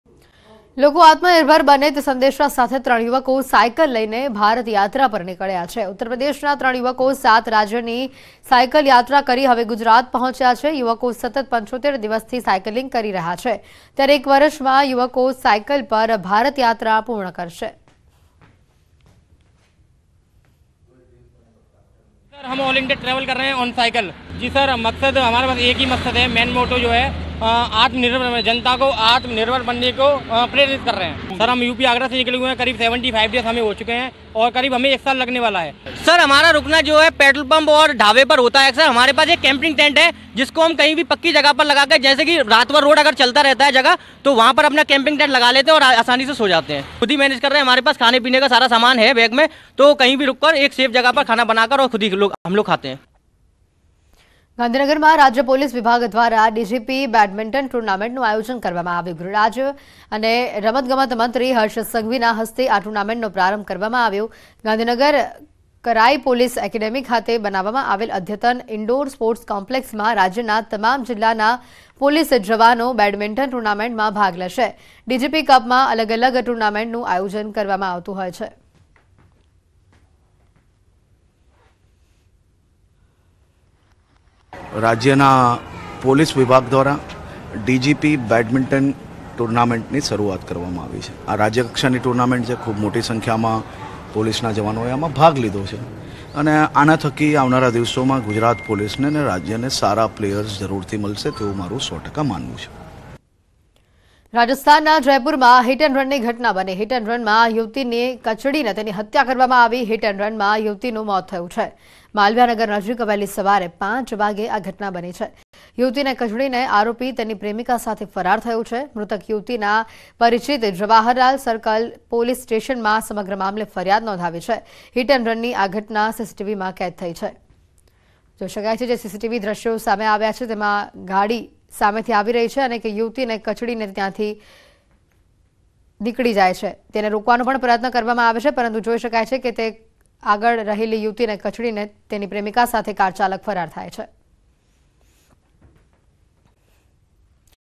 Mehsana News : ત્રણ યુવકો સાયકલ લઈ ભારત યાત્રા પર નીકળ્યા | Bicycle | India Journey | Gujarati News
With the message that people should become self-reliant, three youths have taken a bicycle and set out on a journey to India. Three youths from Uttar Pradesh have reached Gujarat after cycling through seven states.

લોકો આત્મ નિર્ભર બને તે સંદેશા સાથે ત્રણ યુવકો સાયકલ લઈ ભારત યાત્રા પર નીકળ્યા છે. ઉત્તર પ્રદેશના ત્રણ યુવકો સાત રાજ્યની સાયકલ યાત્રા કરી હવે ગુજરાત પહોંચ્યા છે. n18oc_mehsana